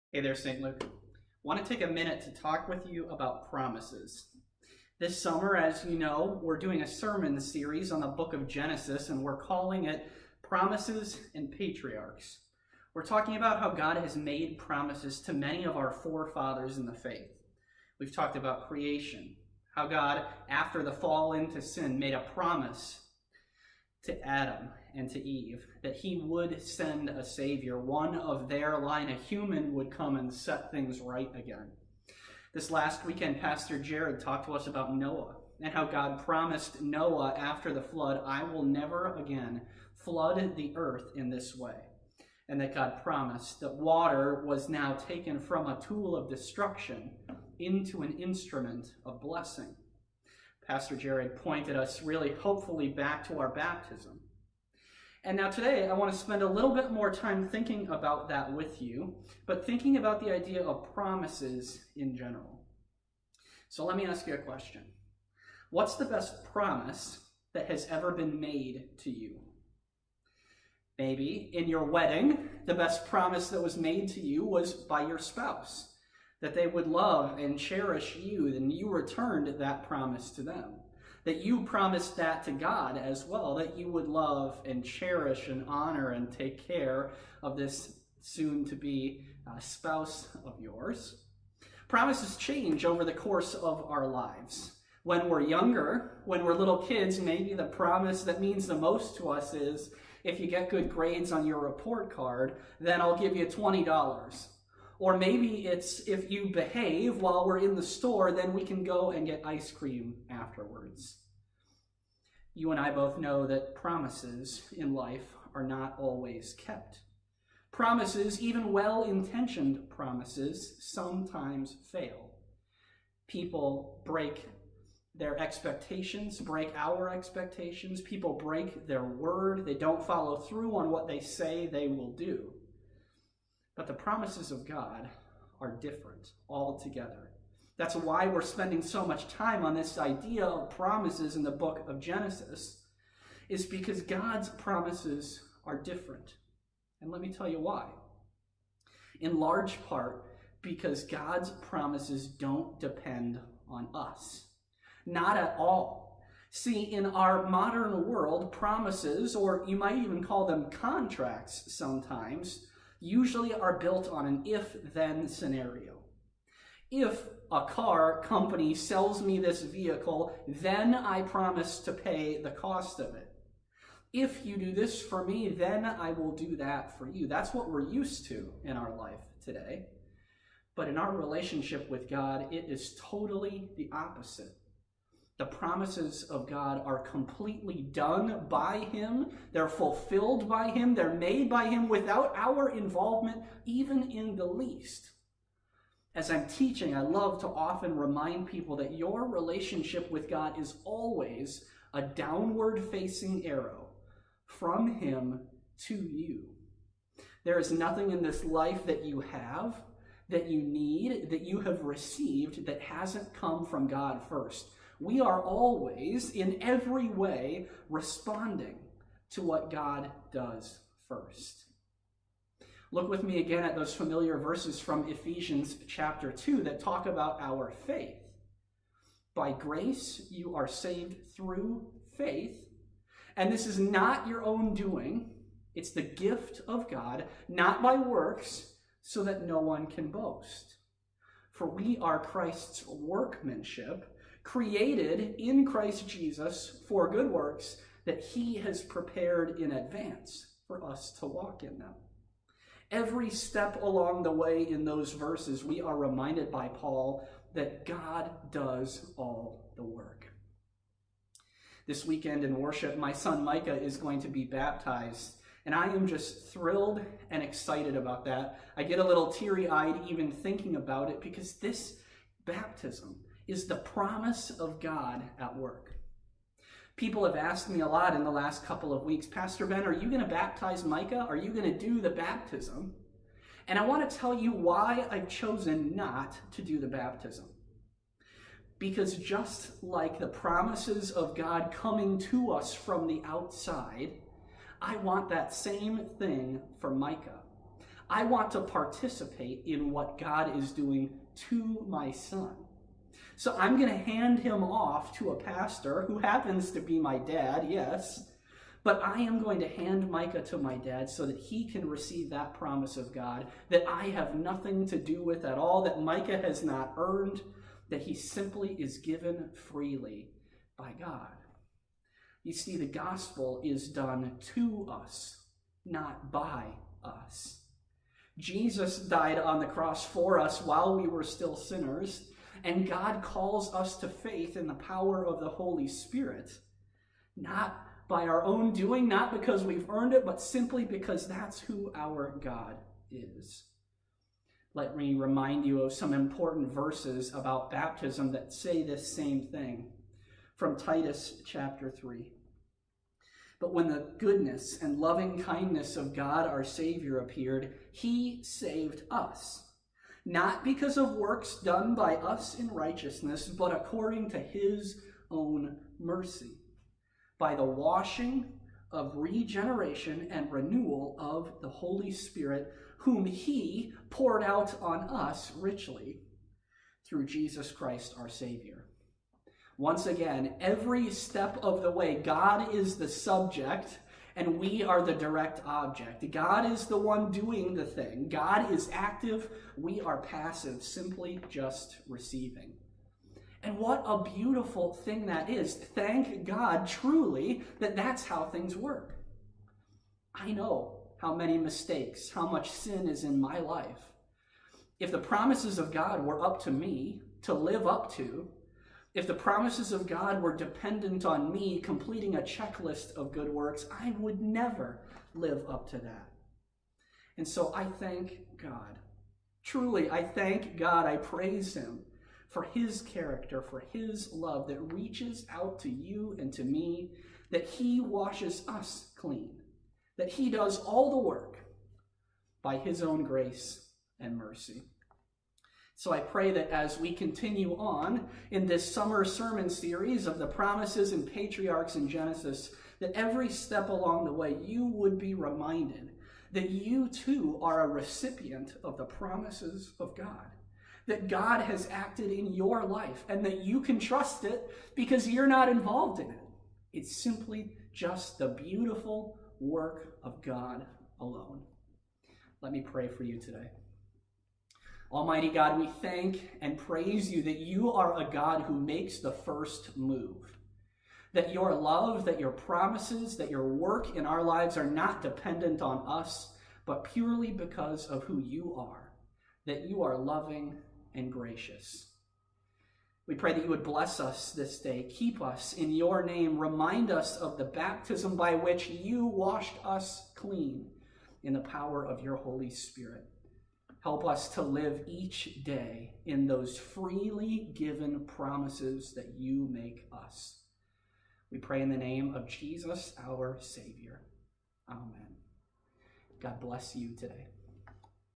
"God's Promises" Daily Devotion for July 1st, 2021
Join Pastor Ben as we think about the reliability of God's promises, and what that has to do with why he's chosen NOT to be the one to baptize his son this weekend! Scriptures from Ephesians 2:8-10 and Titus 3:4-7.